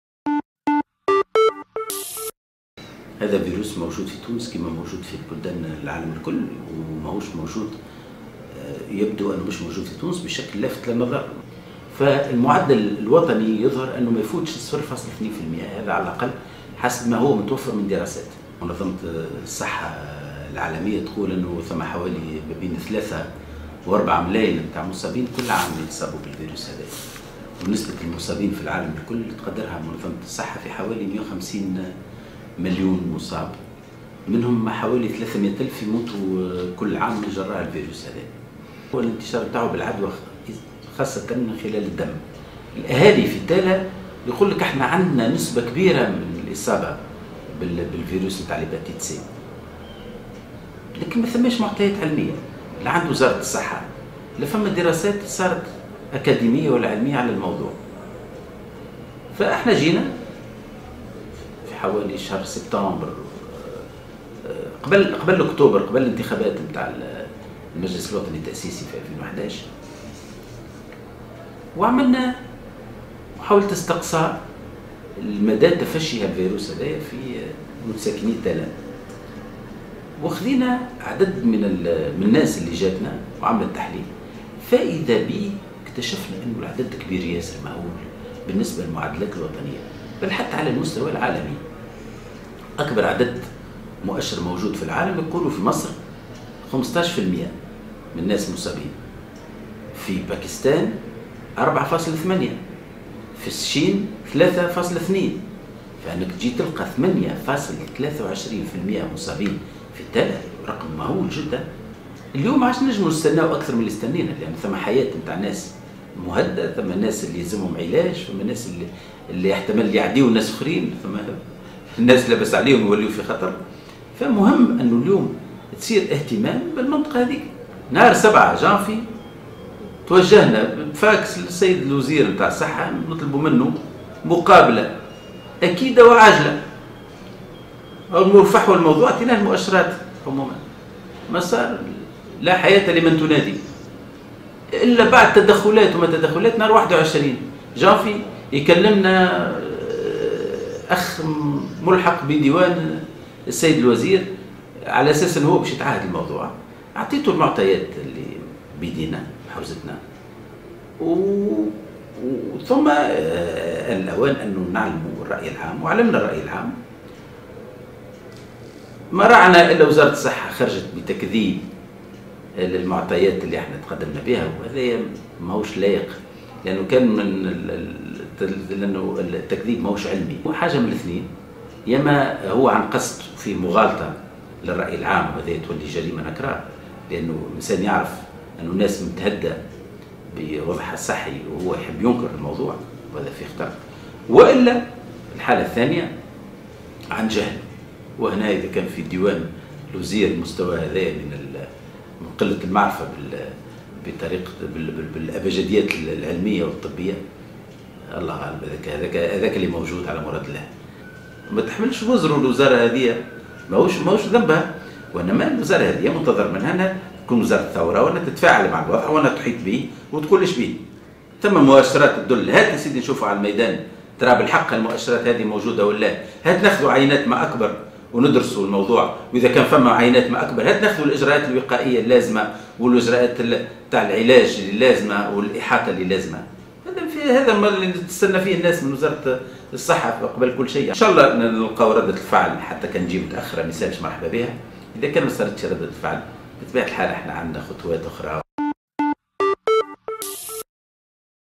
المرايحي: اتصلنا بوزارة الصحة ولم نجد أي رد
الدكتور لطفي المرايحي يتحدث عن انتشار فيروس التهاب الكبد بمعتمدية تالة من ولاية القصرين بنسبة 8.23 بالمائة وردة فعل وزارة الصحة تجاه هذا الموضوع ..
وقد اتصلنا بالدكتور منذر الونيفي المكلف بمهمة لدى وزارة الصحة للاستفسار أكثر حول الموضوع ، وأكد لنا انه طلب من المرايحي تقديم المعطيات التي تثبت صحة هذا الخبر إلا أنه لم يستجب.